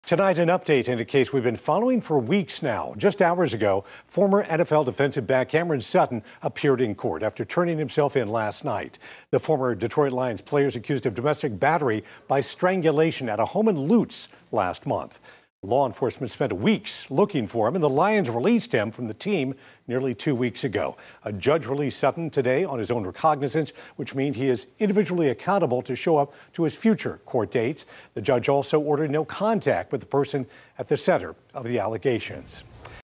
Former NFL player Cam Sutton turns himself in on domestic violence charge, ending weekslong search
Former NFL cornerback Cameron Sutton turned himself in at the Orient Road Jail in Hillsborough County Sunday night, ending a weekslong search.

Sutton, 29, who previously played for the Detroit Lions, was wanted for domestic battery by strangulation.

The Hillsborough County Sheriff's Office (HCSO) asked the public for help finding Sutton back on March 20.

Sutton's attorney told HCSO on March 25 that Sutton planned to turn himself in.

“After weeks of evading law enforcement, this man has finally made the right choice to turn himself in,” Hillsborough County Sheriff Chad Chronister said.

The domestic battery charge stems from an early morning incident at a home in Lutz on March 7, where the sheriff's office responded to a call about domestic violence in progress involving Sutton and a woman.

The Detroit Lions released Sutton following the allegations on March 21.

Detroit signed Sutton to a $33 million, three-year contract a year ago. He helped the franchise win a division title for the first time in three decades along with two playoff games in one postseason for the first time since 1957.

He started in all 17 regular season games, making a career-high 65 tackles and one interception, and in each of Detroit’s three playoff games.

He was drafted by the Pittsburgh Steelers in 2017.

Domestic battery by strangulation is a third-degree felony punishable by up to five years in prison.

Sutton made his first appearance before a Hillsborough County judge at 1:30 p.m. Monday. Officials from the 13th Judicial Court told 10 Tampa Bay he was released on recognizance with a no violent contact order.